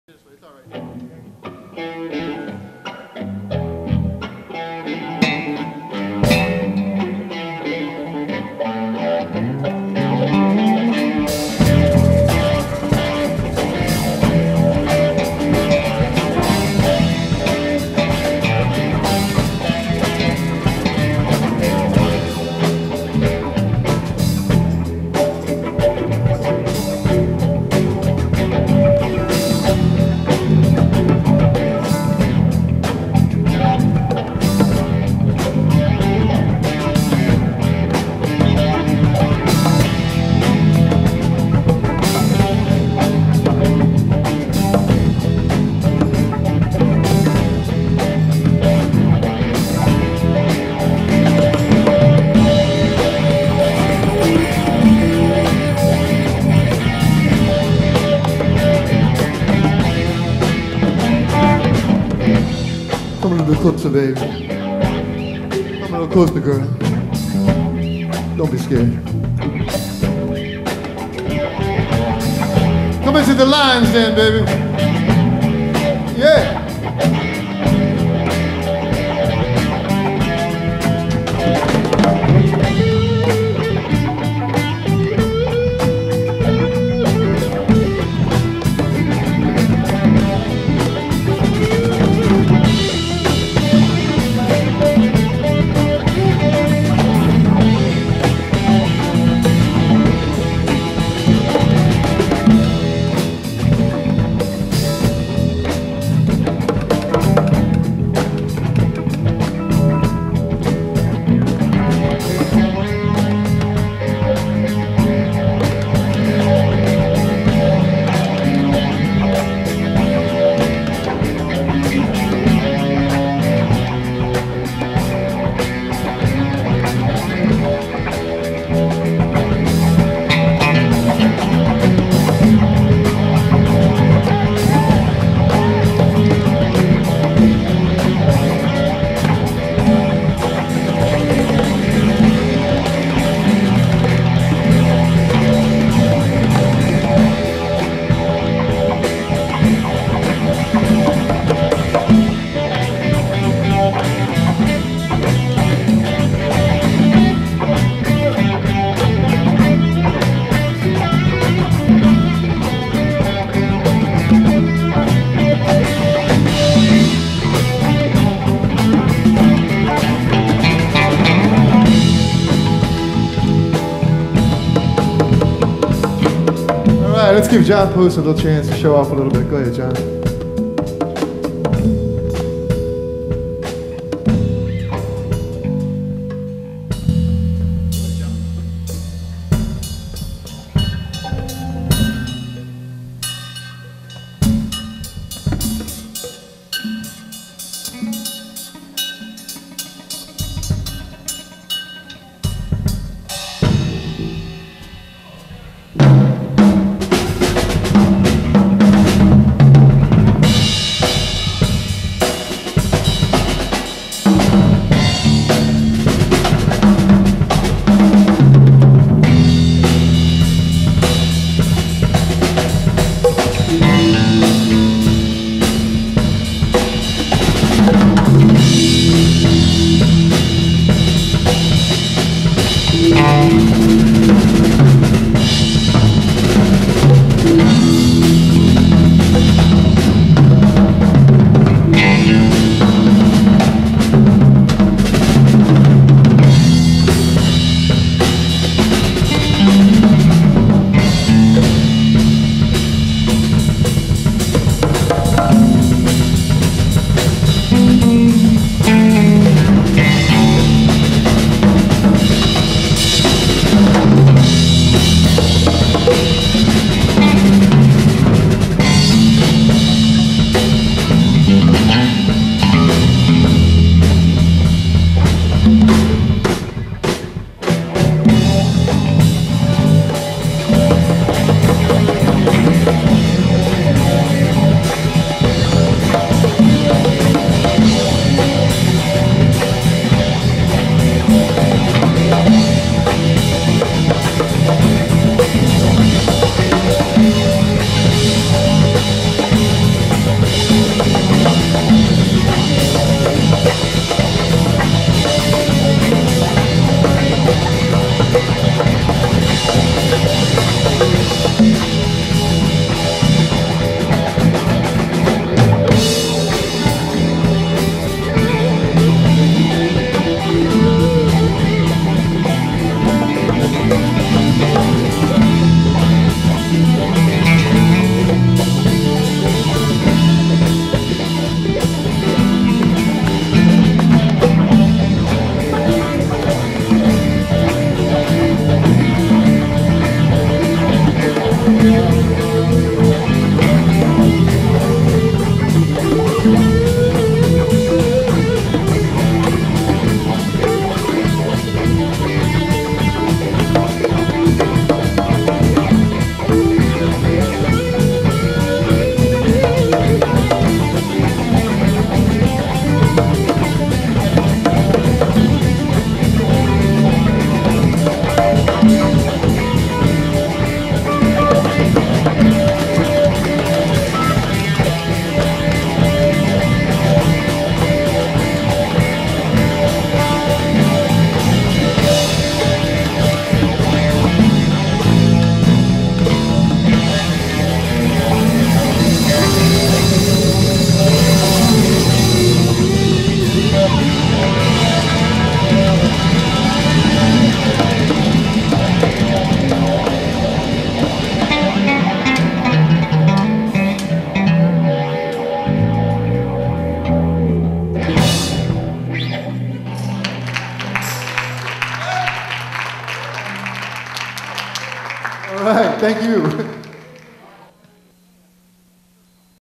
The Lions Den
Bowie Center for the Performing Arts - Local Artists Series, Sunday, November 16, 2014. Stu Gray (guitar), Mark Ingino (guitar), Rowe Oliver (bass), Steve Abramowitz (bass), John Post (drums), Herbie Wheatley (congas), Jason Carzon (percussion).